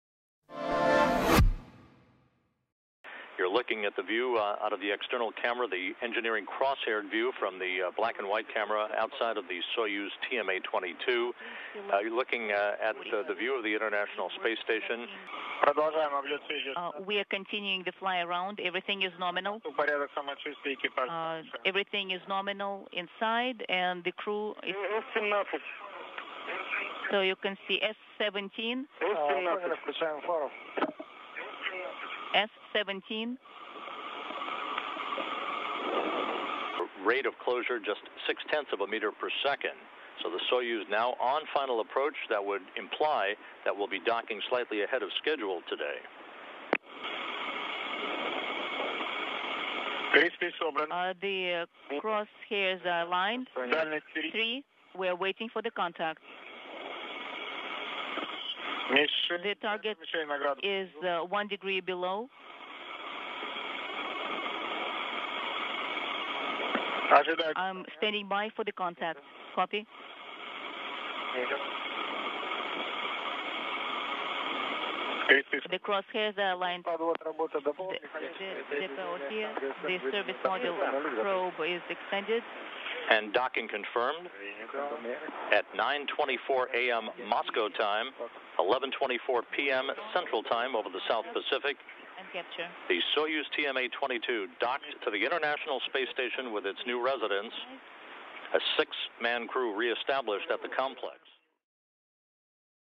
Soyuz capsule docks with International Space Station
A Russian Soyuz capsule successfully docked at the International Space Station to deliver a new crew to the floating space lab.